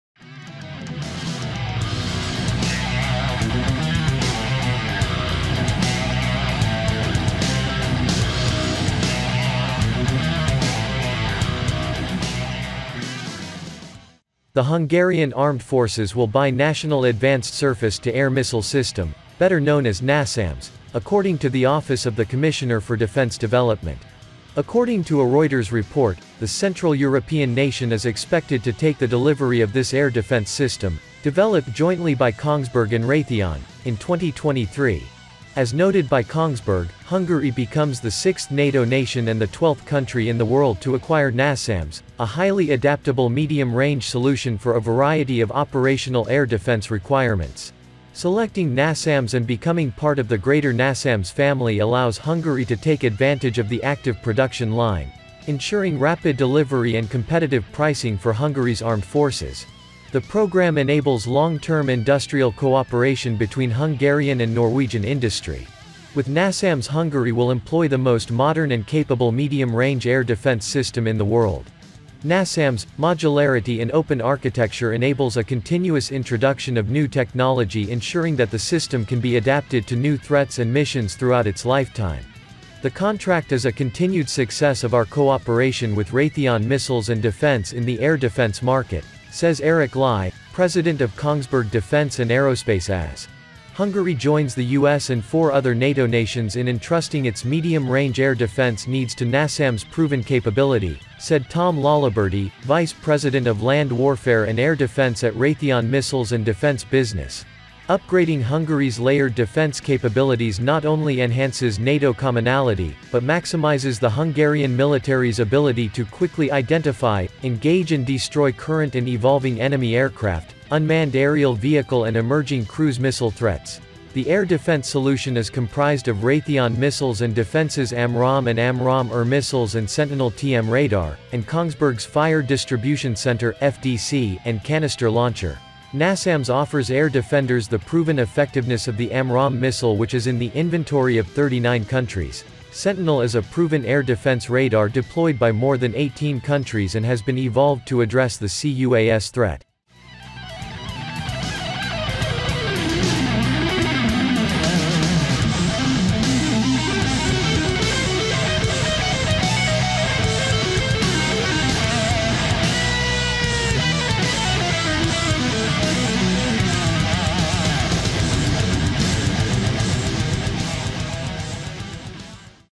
Hungarian Armed Forces selects NASAMS air defense system
The Hungarian Armed Forces will buy National Advanced Surface to Air Missile System, better known as NASAMS, according to the office of the commissioner for defense development.

According to a Reuters report, the Central European nation is expected to take the delivery of this air defense system, developed jointly by Kongsberg and Raytheon, in 2023.

As noted by Kongsberg, Hungary becomes the 6th NATO nation and the 12th country in the world to acquire NASAMS, a highly adaptable medium-range solution for a variety of operational air defense requirements.

Selecting NASAMS and becoming part of the greater NASAMS family allows Hungary to take advantage of the active production line – ensuring rapid delivery and competitive pricing for Hungary’s armed forces. The program enables long term industrial cooperation between Hungarian and Norwegian industry.

“With NASAMS Hungary will employ the most modern and capable medium-range air defense system in the world. NASAMS’ modularity and open architecture enables a continuous introduction of new technology ensuring that the system can be adapted to new threats and missions throughout its lifetime. The contract is a continued success of our cooperation with Raytheon Missiles & Defense in the air defense market,” says Eirik Lie, President of Kongsberg Defence & Aerospace AS.

“Hungary joins the U.S. and four other NATO nations in entrusting its medium-range air defense needs to NASAMS proven capability,” said Tom Laliberty, vice president of Land Warfare & Air Defense at Raytheon Missiles & Defense business. “Upgrading Hungary’s layered defense capabilities not only enhances NATO commonality, but maximizes the Hungarian military’s ability to quickly identify, engage and destroy current and evolving enemy aircraft, unmanned aerial vehicle and emerging cruise missile threats.”

The air defense solution is comprised of Raytheon Missiles & Defense’s AMRAAM and AMRAAM-ER missiles and SentinelTM radar, and Kongsberg’s Fire Distribution Center (FDC) and canister launcher. NASAMS offers air defenders the proven effectiveness of the AMRAAM missile which is in the inventory of 39 countries. Sentinel is a proven air defense radar deployed by more than 18 countries and has been evolved to address the C-UAS threat.